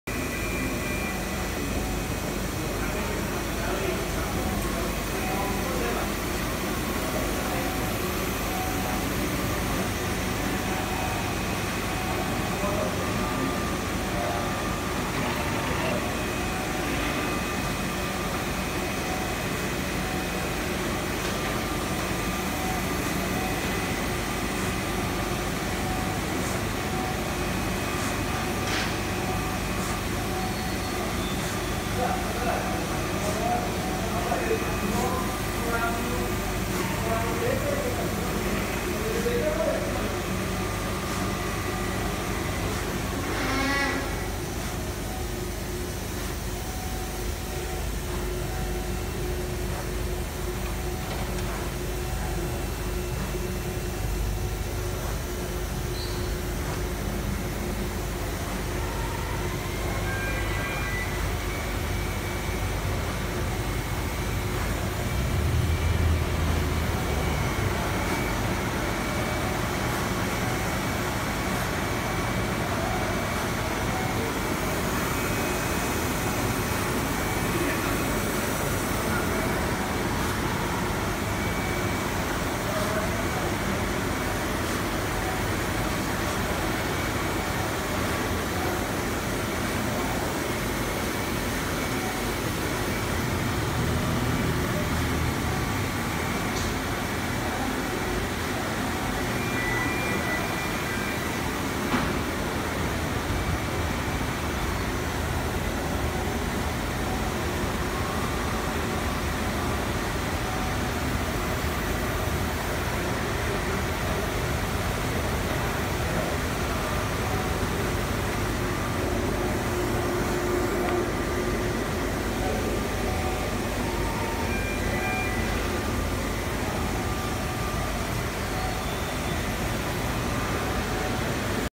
Printer sublim terbaru 2019 hp stitch S500 64 inc, plotter sublim hp hub AKIRADATA 081310422455
Printer sublim terbaru 2019 dari hp, hp stitch S500 64 inc , cost tinta lebih murah dan irit dengan Tekhnologi thermal inkjet lebih irit tinta , harga tinta terjangkau , printer sublim dari hp ini memiliki tinta dengan kapasitas tinta 3 liter , cosh tinta sangat murah dan speed printer ini juga sangat cepet , hp stitich S500 memiliki 4 Warna cmyk , inc software rip , Untuk tes print dan Penjualan bisa menghubungi kami AKIRADATA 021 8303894 Atau phone dan whassap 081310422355